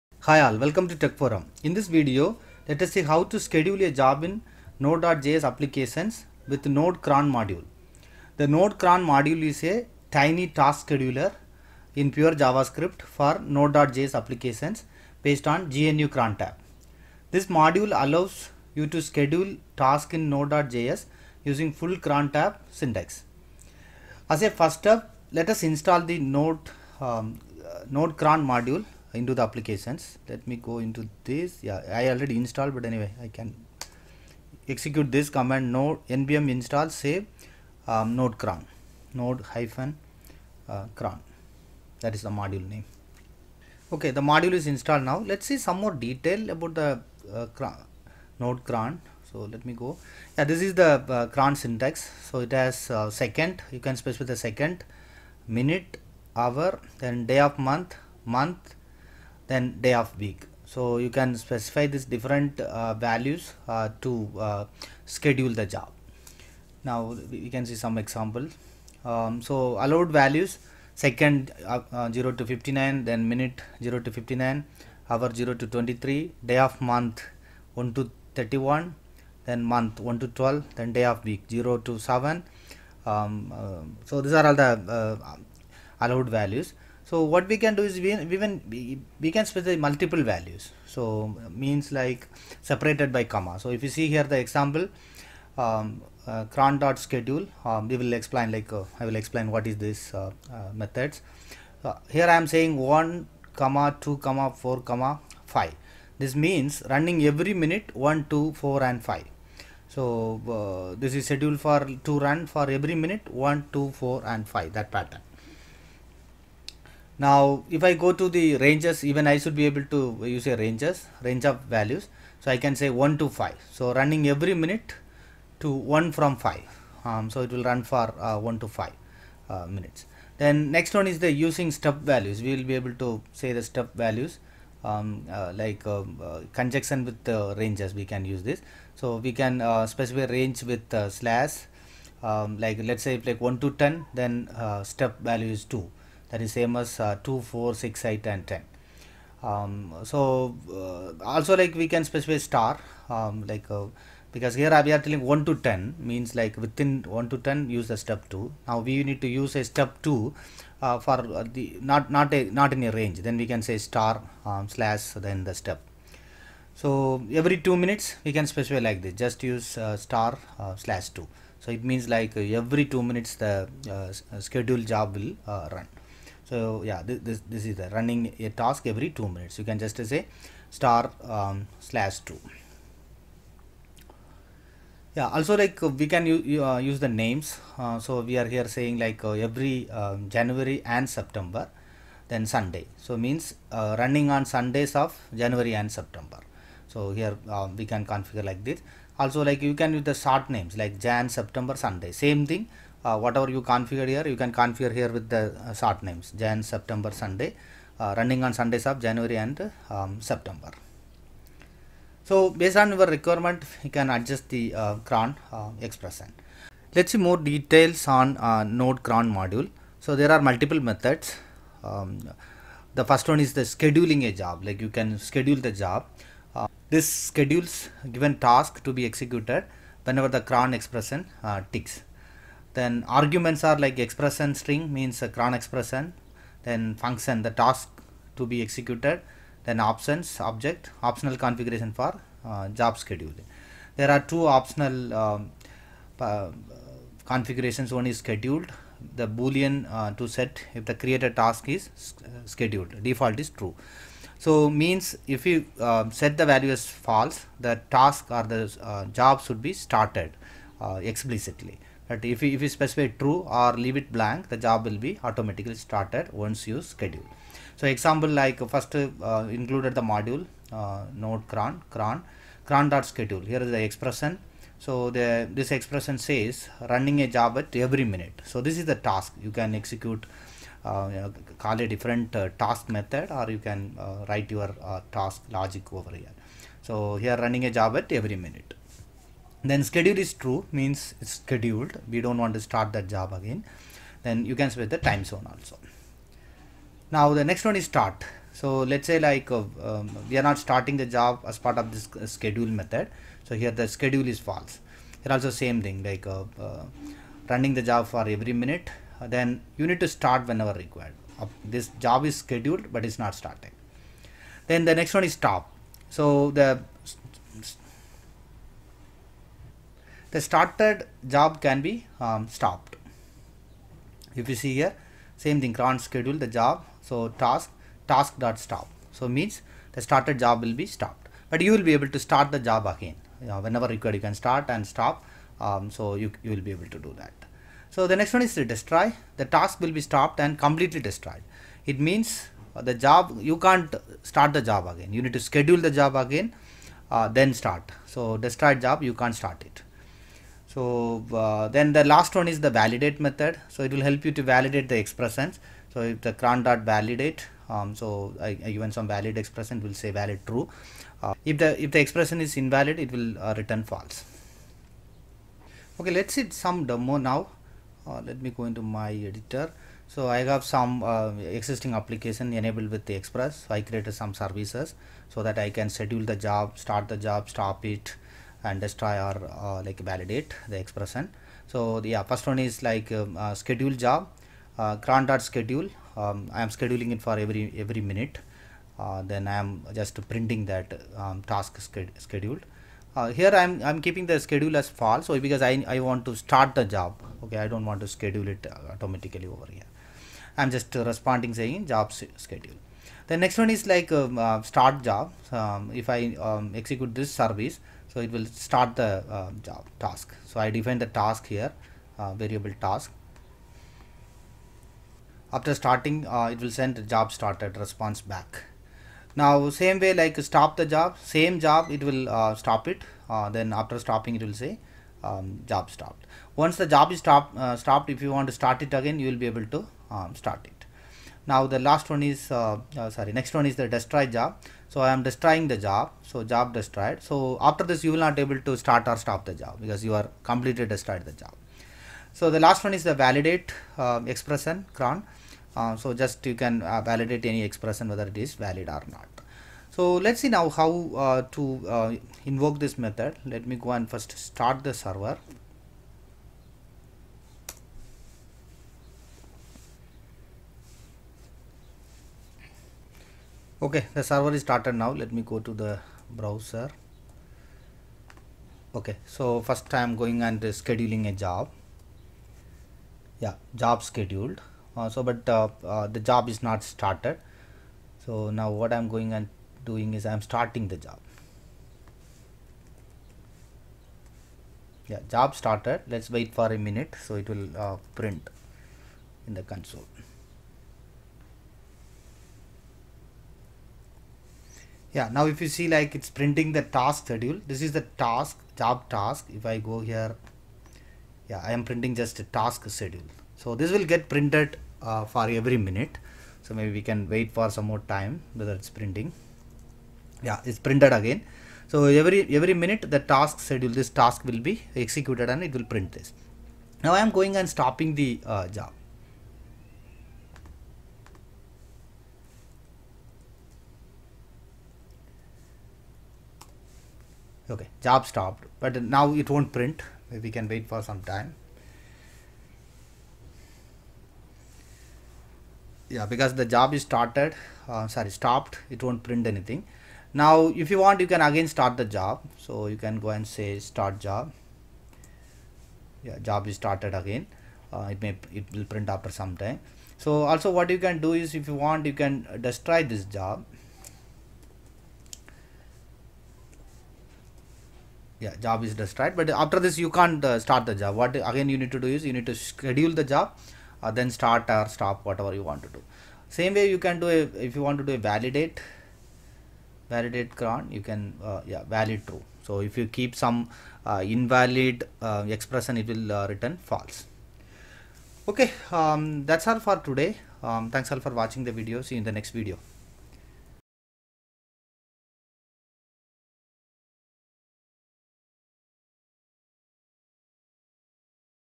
How to schedule Job(Tasks) in Node.js with node-cron
The node-cron module is a tiny task scheduler in pure JavaScript for node.js based on GNU crontab. This module allows you to schedule tasks in node.js using full crontab syntax.

References:
@/node-cron
@um-repo/youttubedata/blob/master/node-cron/JobHandler.js